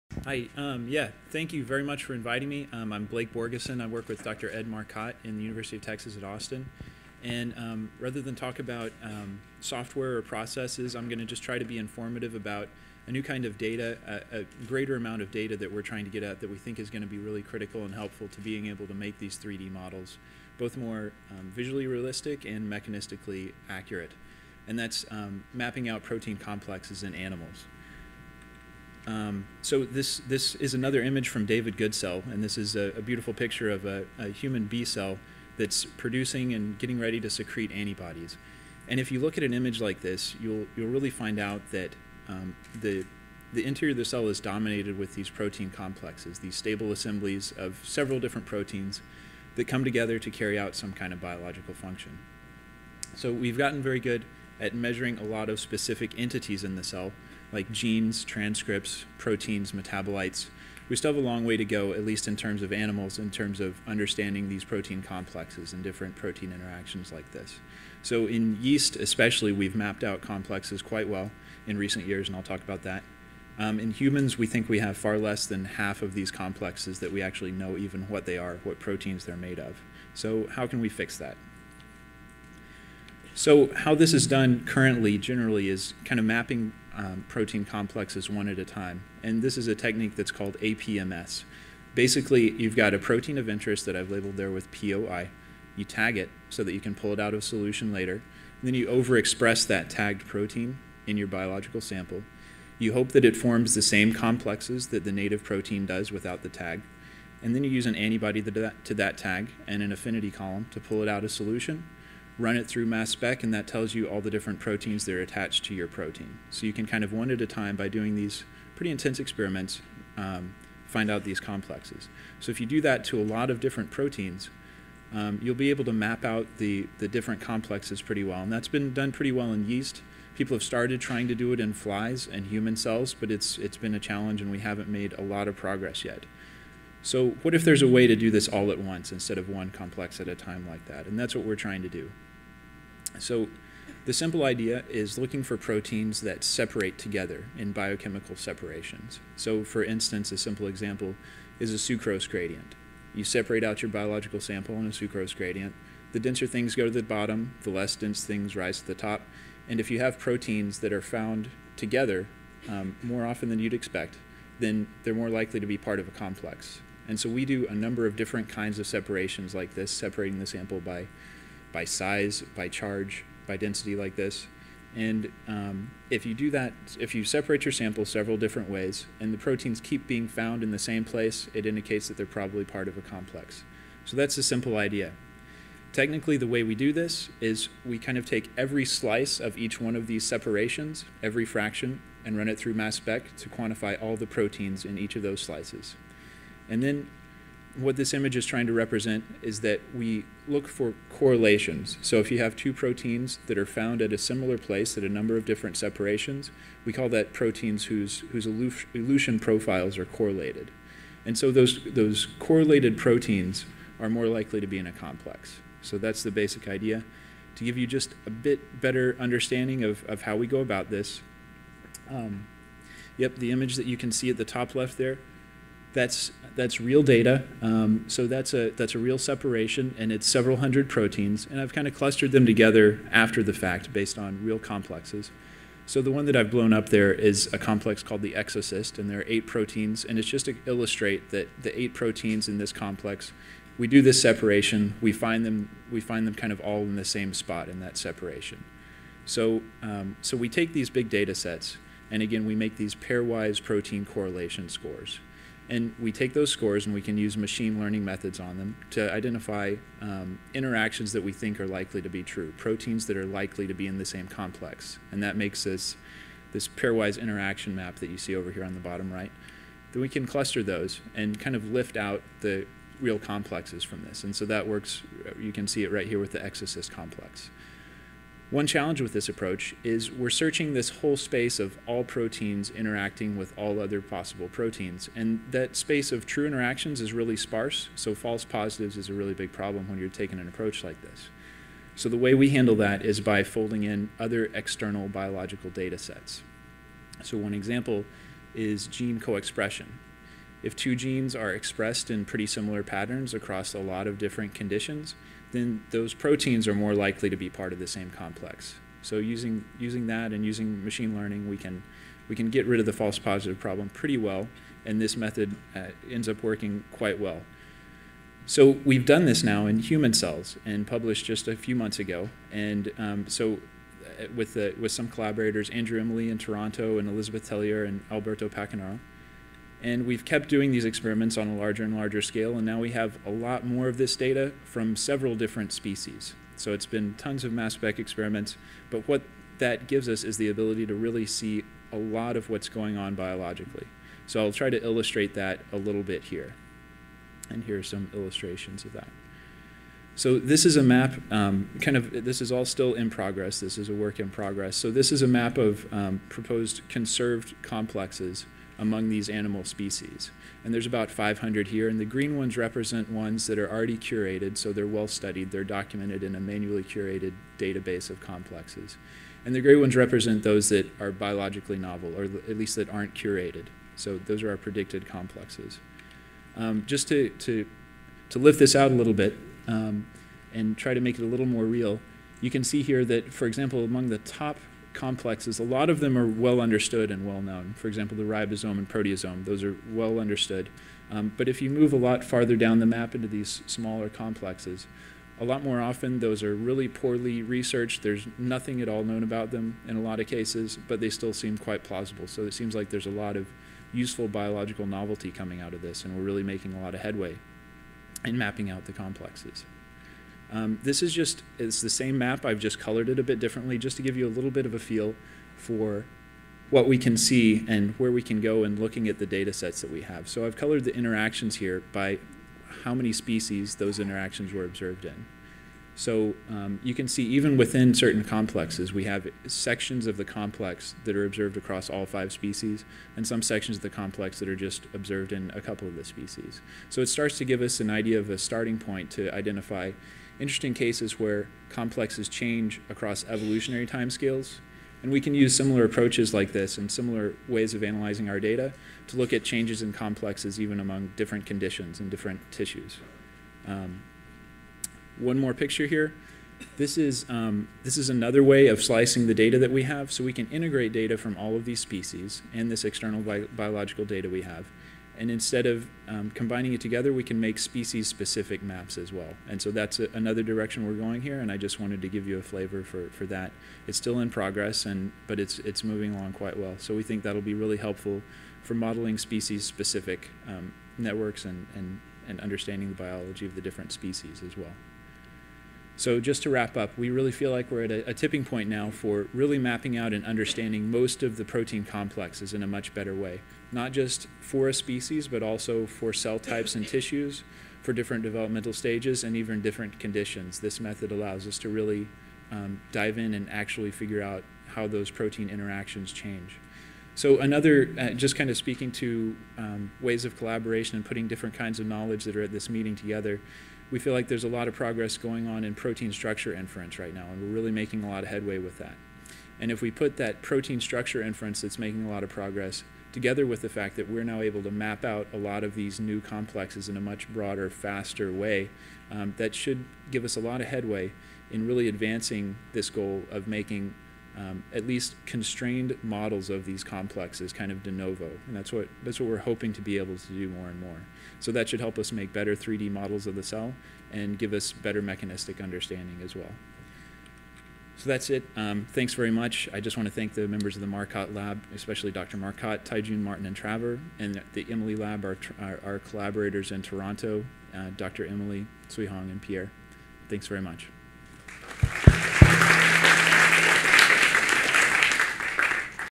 Mapping Animal Protein Complexes: Scaling Up - Blake Borgeson
Toward the 3D Virtual Cell Conference, December 13-14, 2012 - San Diego
Mapping Animal Protein Complexes: Scaling Up - Blake Borgeson, University of Texas at Austin